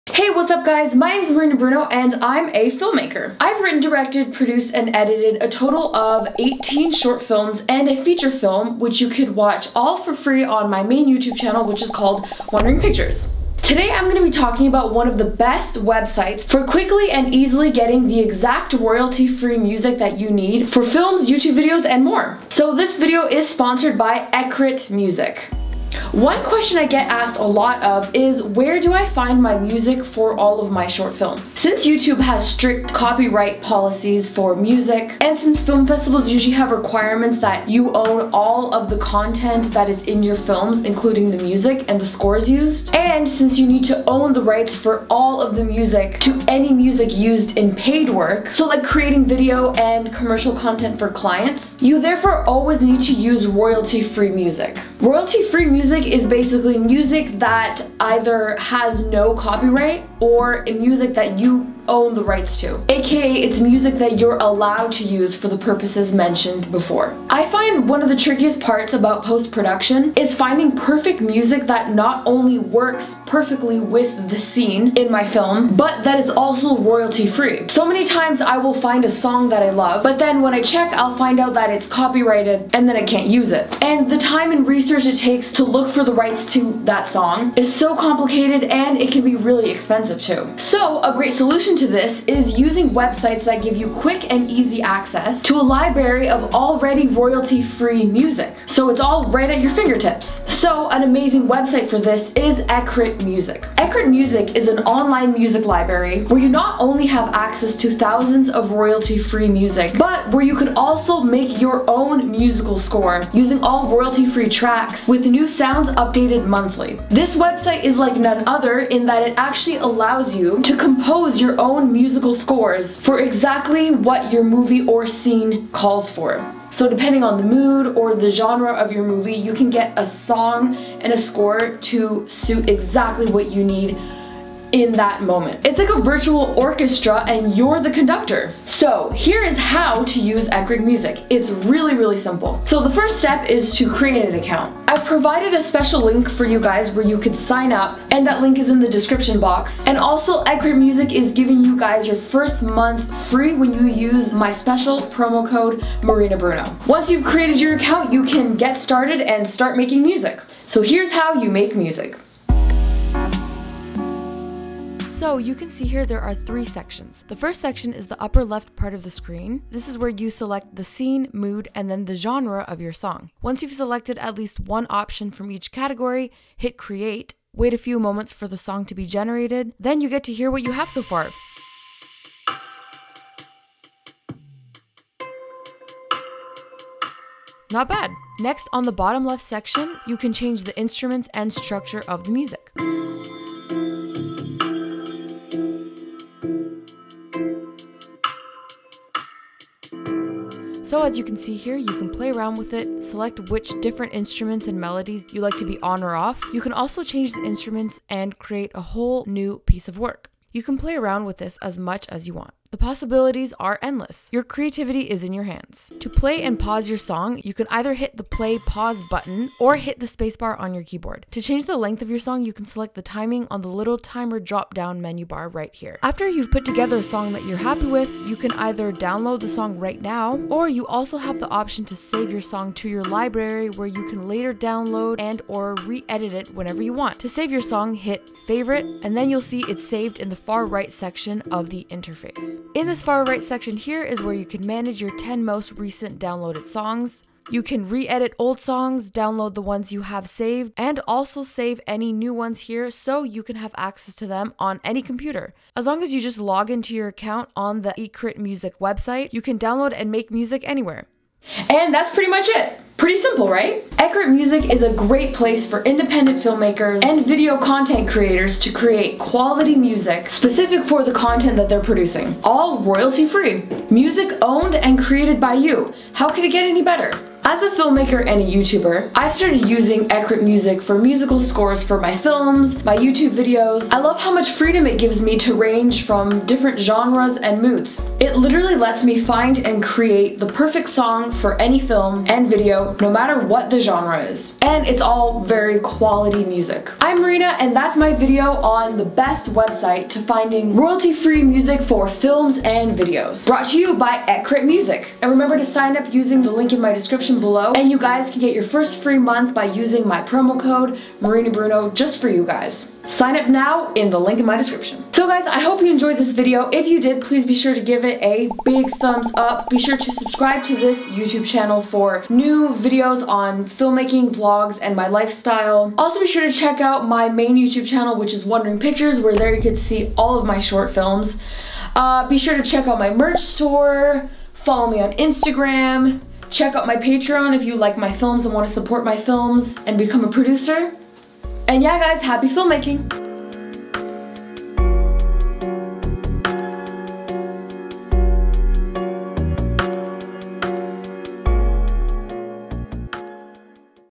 Best Website for ROYALTY FREE MUSIC for YouTube Videos & Film | Ecrett Music
Ever wondered where to get copyright free music to use for you YouTube videos, short films & client work? Well look no further! Ecrett Music is the best website for not only having access to thousands of royalty free songs and tracks you can use (updated monthly) but where you can MAKE your own songs to match the exact genre, mood & tempo you need.

GEAR I USE ↓
Gorilla Tripod